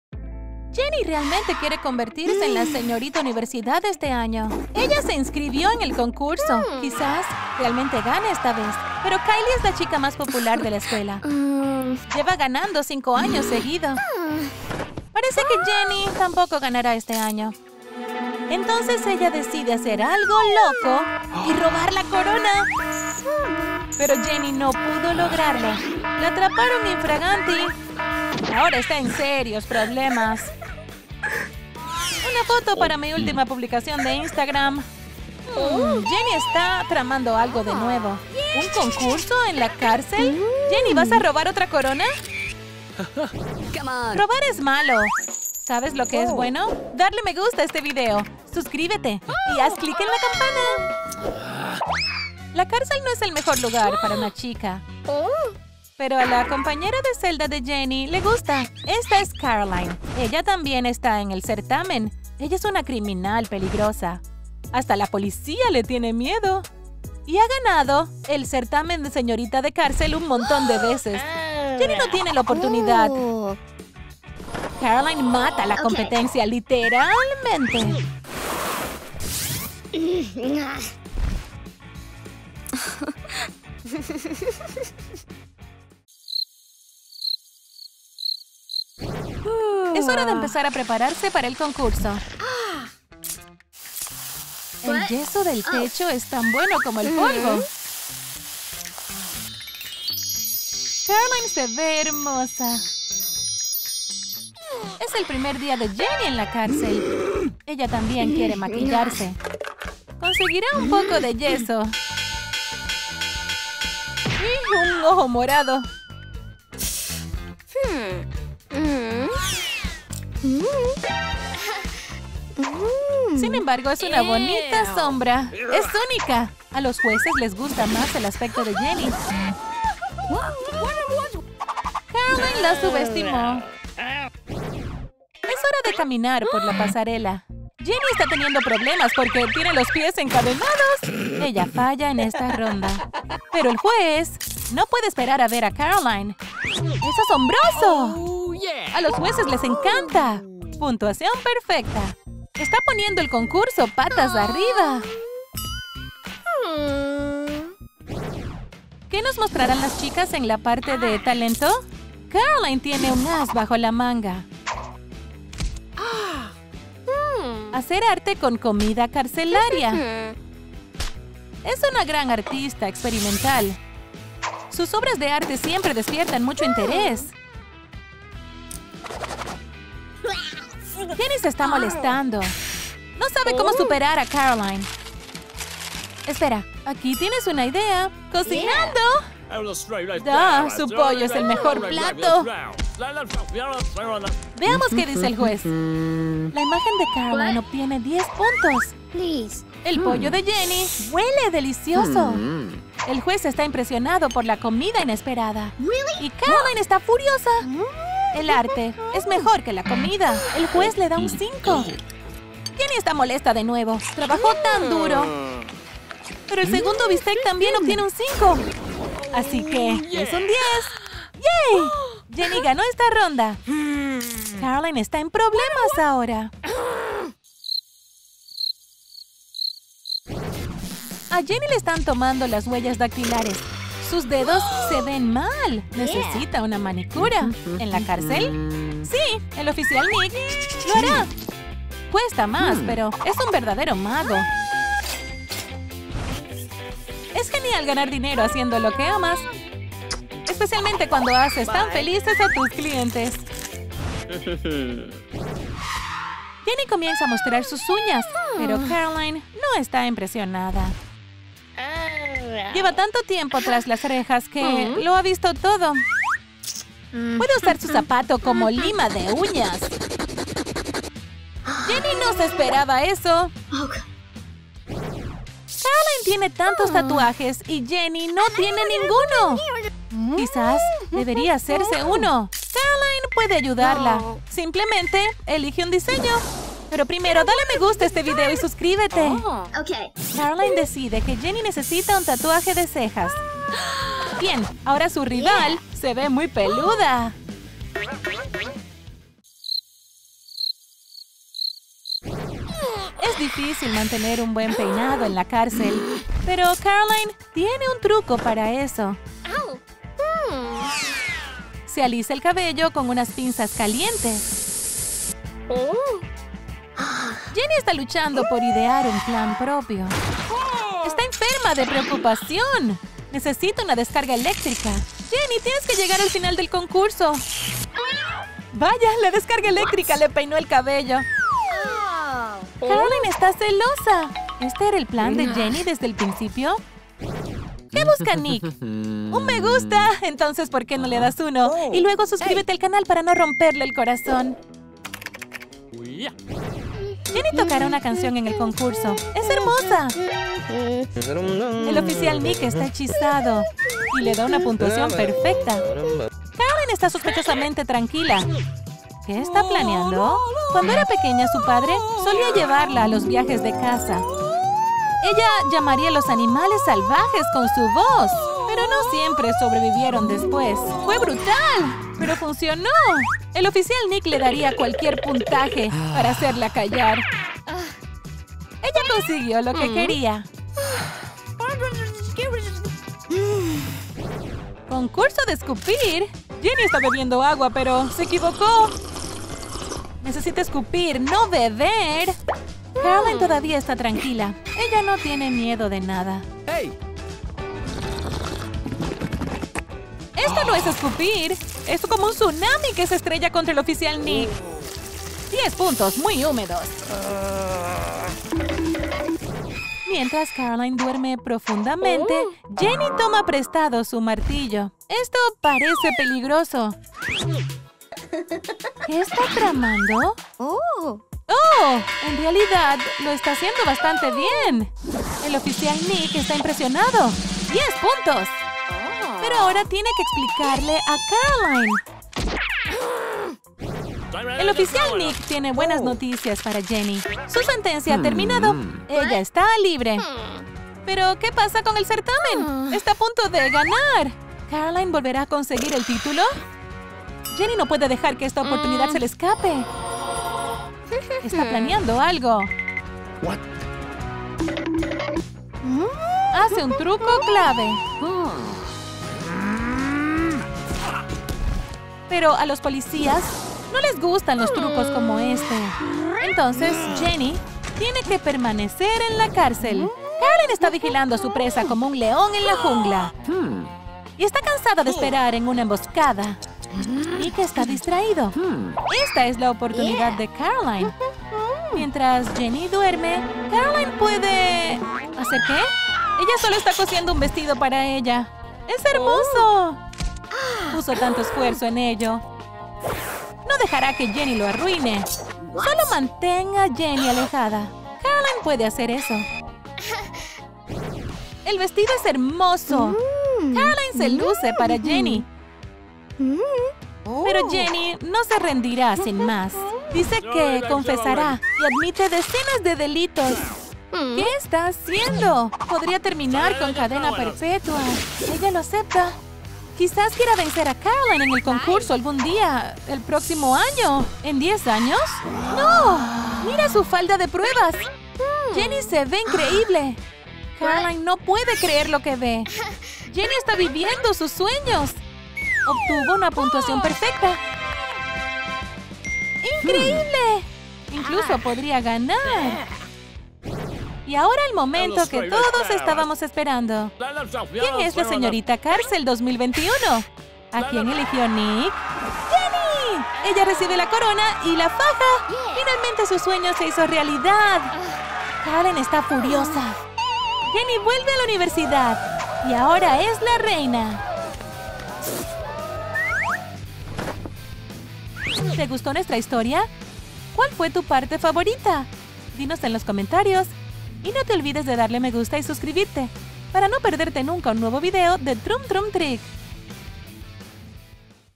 ¡Cómo Hacerse Popular En La Cárcel! / ¡Popular vs Perdedor!
Incluso en la cárcel tienes que luchar por un lugar al sol. Vea ideas divertidas sobre cómo hacerse popular en la cárcel en nuestro nuevo video.